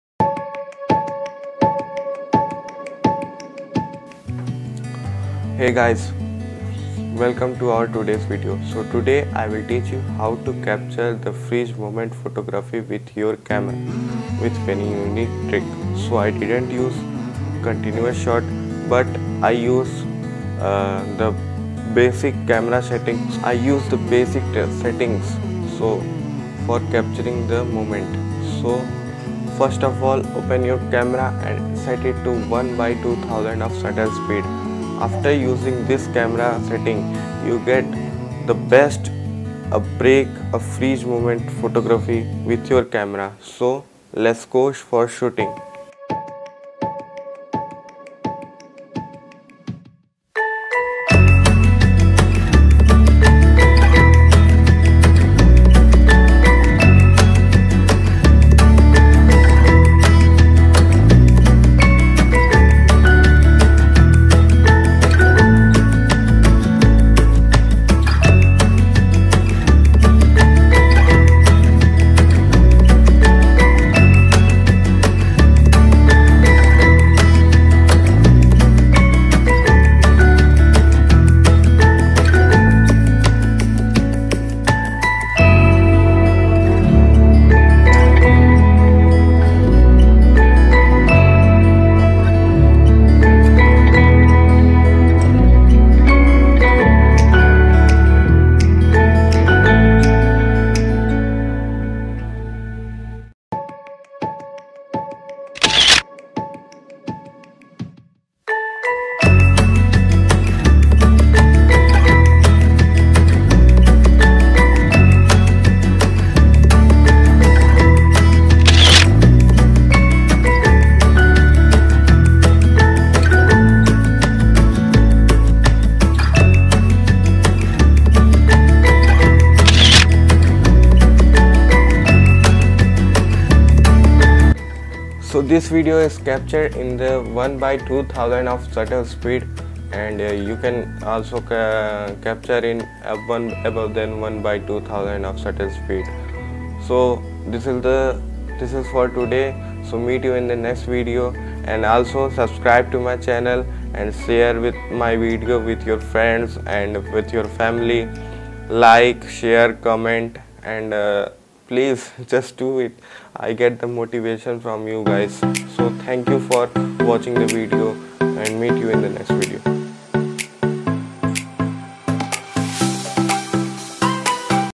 How to capture the momment?| How to freeze the momment?
Hii guys this is my ninth video .
In this video I capture the momment and freeze the momment with very accurate click you can do without going for continuous shot capture without filling your SD Card go and let's be creative .
You can also shoot in phone if you want phone freeze momment then please comment and say"make video of freezing the momment with phone ...
Gear which I used in this video :-
I use no copyright song from vlog music